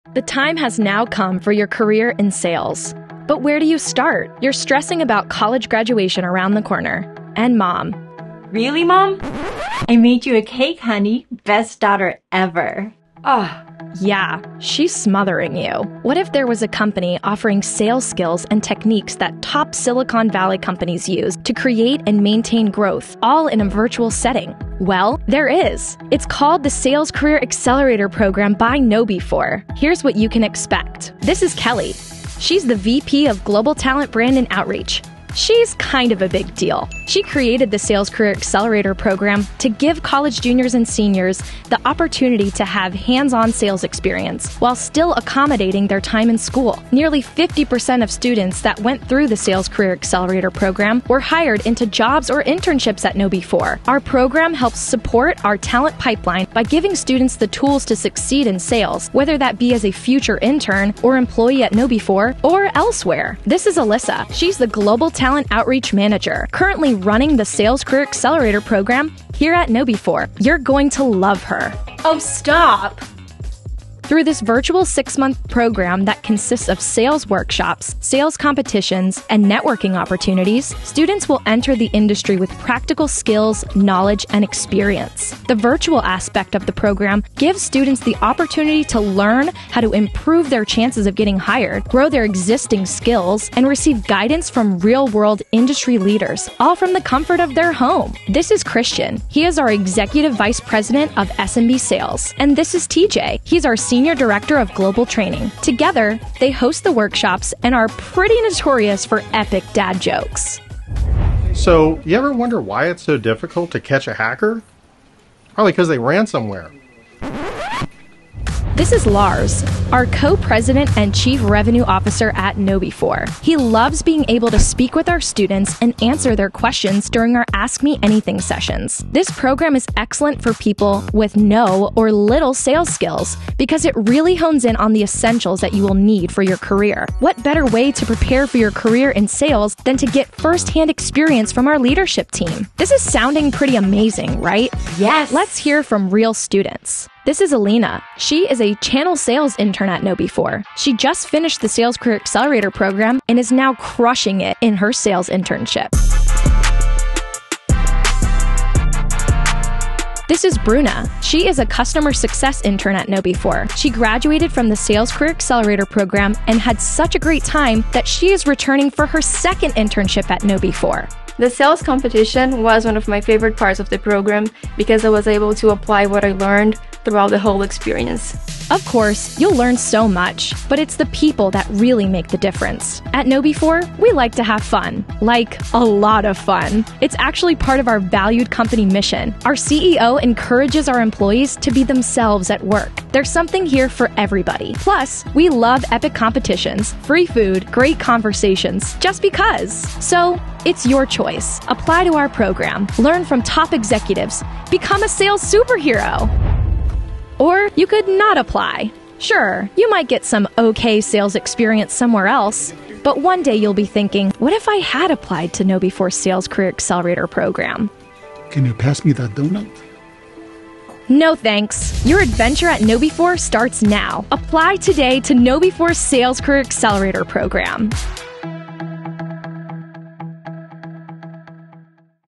KnowBe4's Sales Career Accelerator Program
KnowBe4’s Sales Career Accelerator is a 6-month virtual training and professional development program for college Juniors and Seniors that are interested in pursuing a career in sales. Through this program, students will enter the industry with practical skills, knowledge, and experience.

Please note that this program is 100% virtual, is informational in nature, and does not carry any job duties or responsibilities. Participants will not be paid. Participation in the program does not guarantee any future employment with KnowBe4.